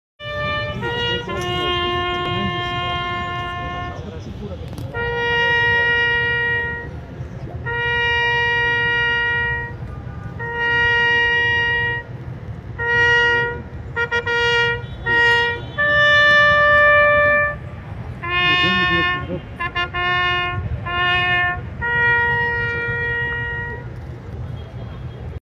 L'attesa del Presidente Sergio Mattarella ai festeggiamenti per le Quattro Giornate di Napoli
NanoTV - Le prove per l'attesa del Presidente SergioMattarella ai festeggiamenti 80 anni per le QuattroGiornate di Napoli

Hai notizie o video da segnalarci? Invia un messaggio alla nostra pagina Facebook